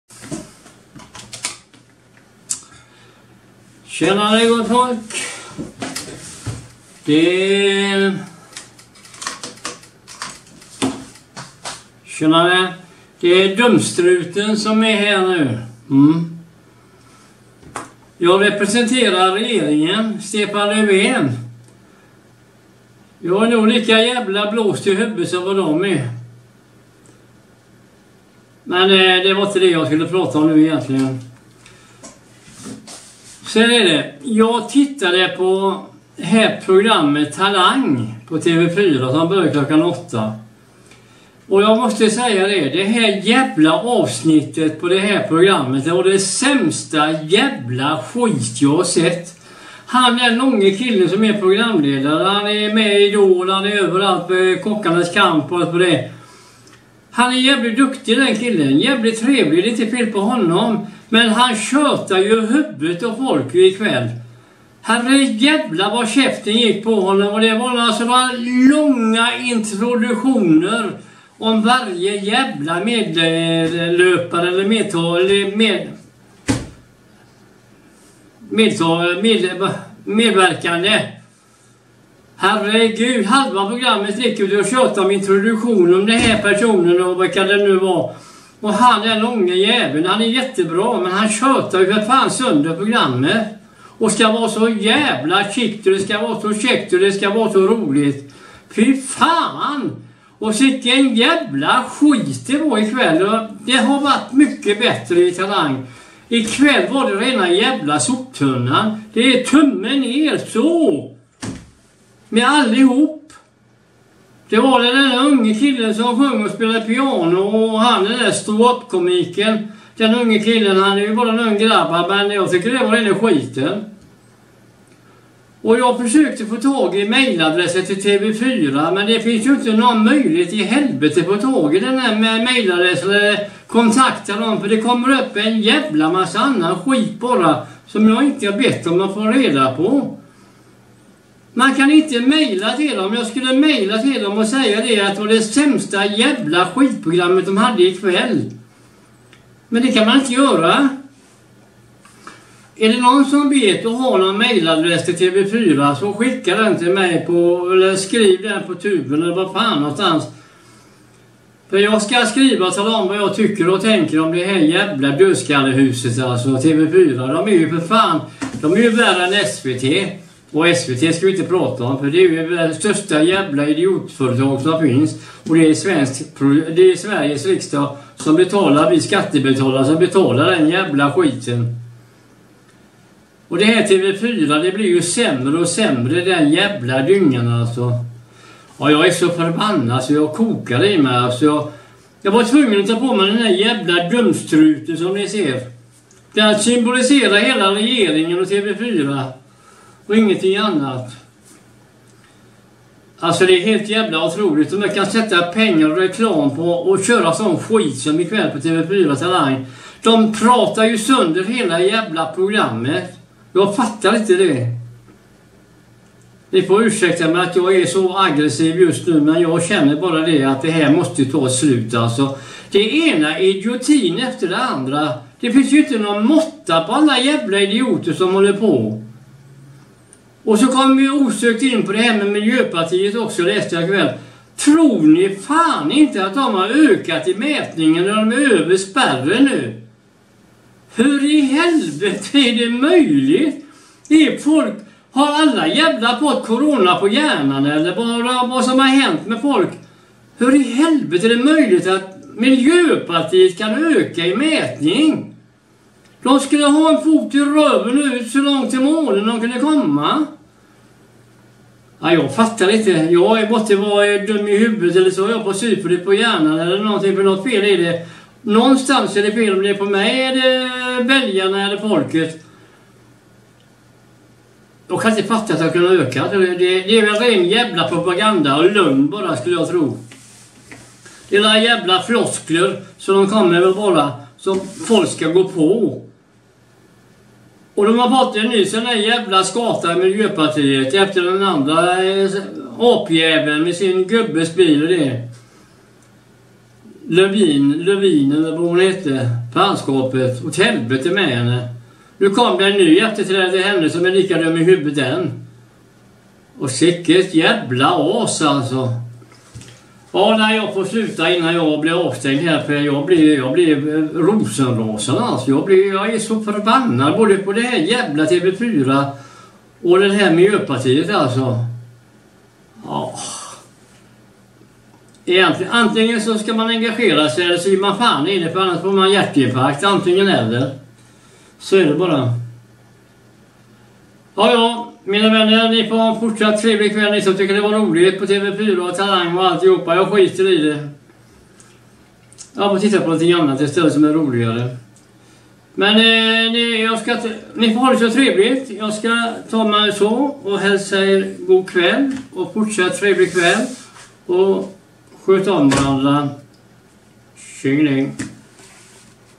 Dumstruten på
Blir så förbannad på TV4 ikväll och Talang. Sådant skit det var ikväll & en programledare som tjötade sönder hela skiten. Åt helvete med allt vad TV4 heter